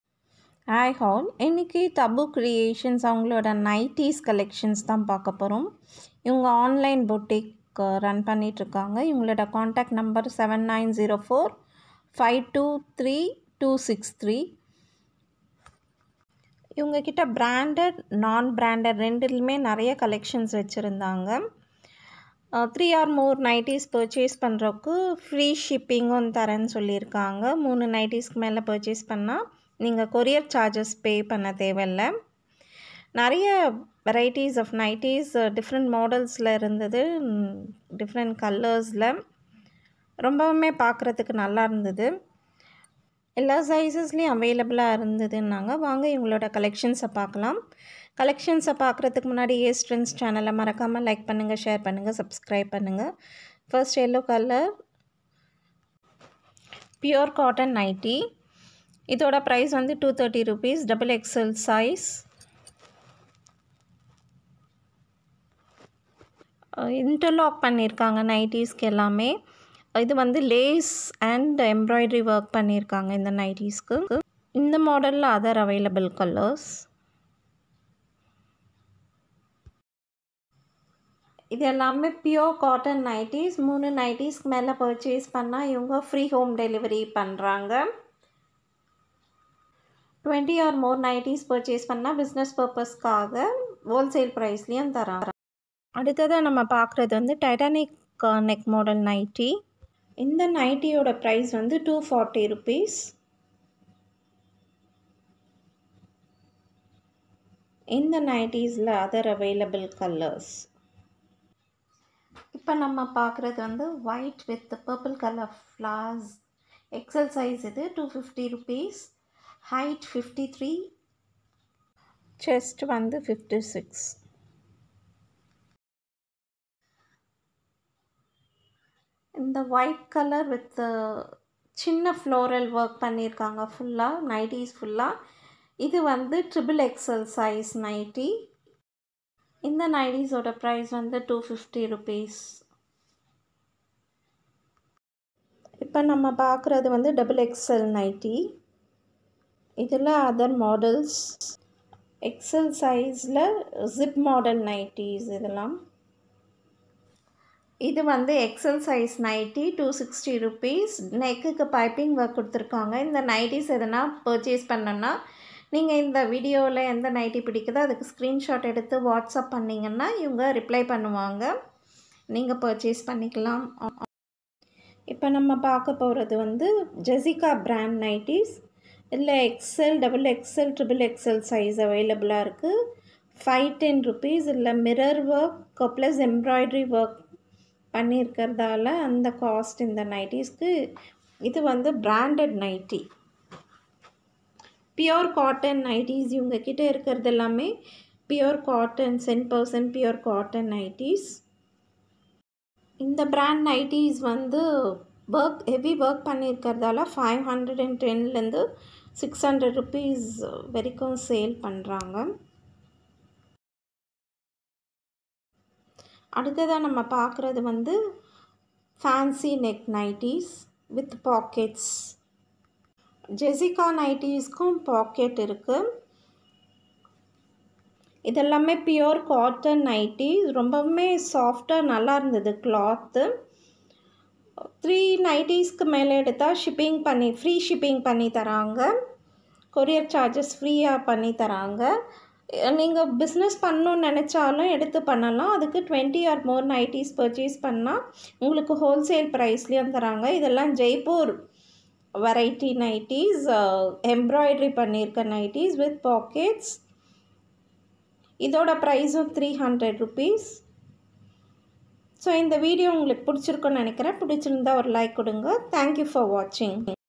Trendy Nighties Collections 2021 | Tabu Creations nighties | Business Opportunity for Ladies
Trendy Nighties Collections 2021 | Tabu Creations nighties wholesale and retail | Business Opportunity for Women
This video is about nighties collections from Tabu Creations. She is selling nighties on Wholesale and retail. She has huge Collections of nighties both branded and no branded. Nighties are at very affordable price . she has variety of collections of fancy jewellery items. Since nighties are most comfortable dress to work at time. it is likely bought by women . It is a comfortable dress to wear at home
Tabu Creations